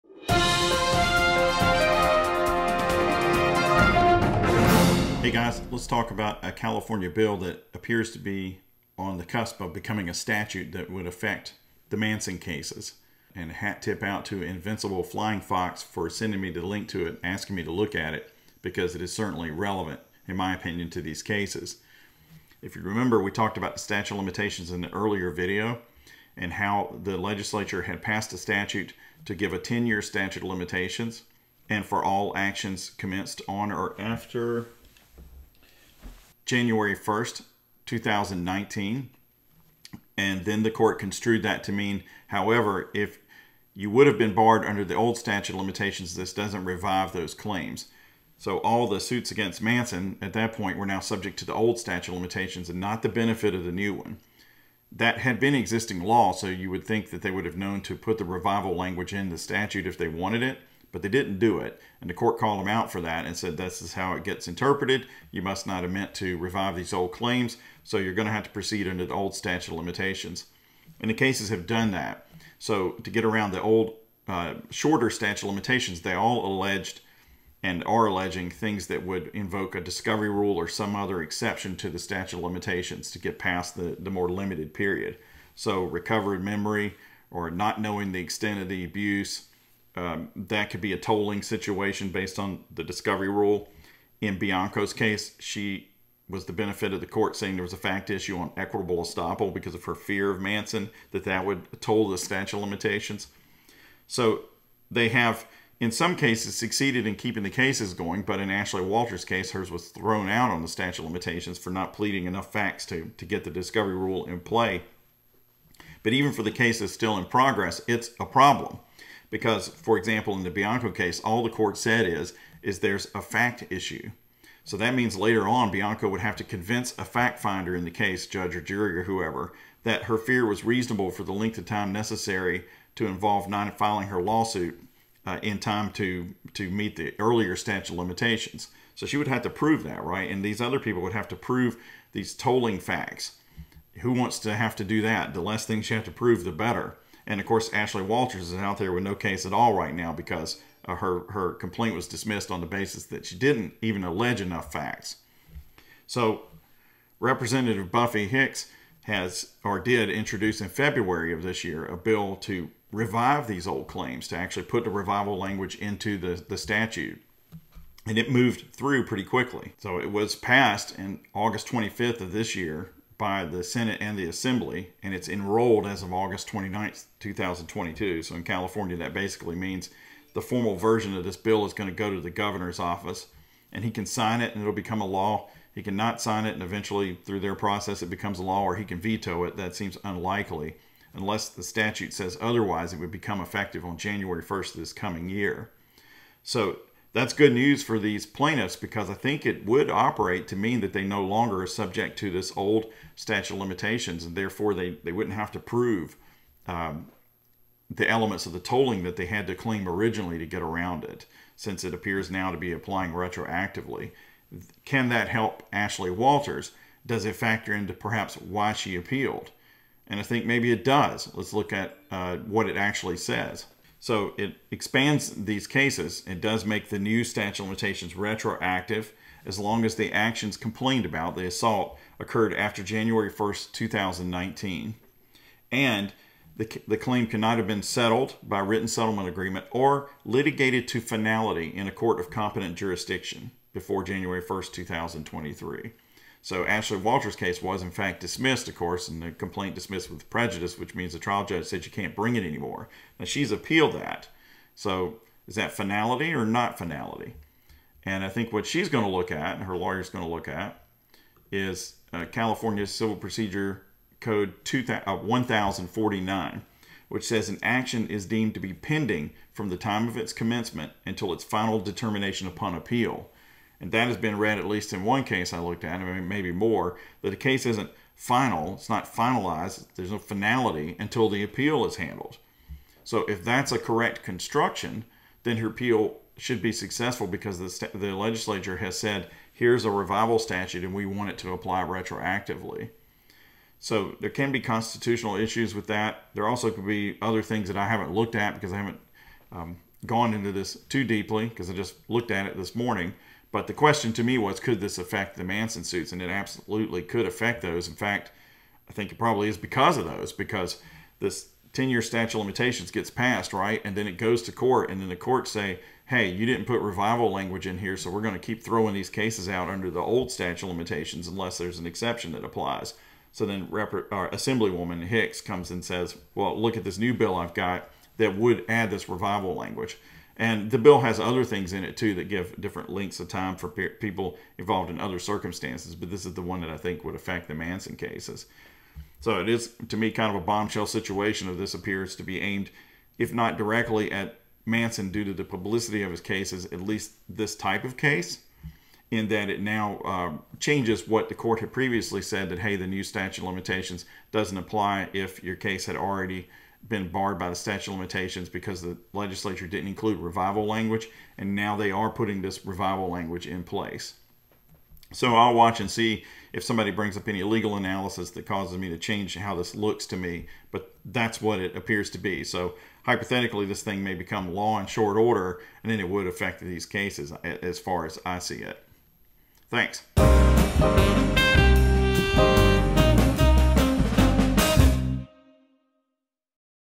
Breaking: New California Law Aimed at the Manson Cases?
Will a new California law make all the plaintiffs' statute of limitation worries go away?? marilynmanson.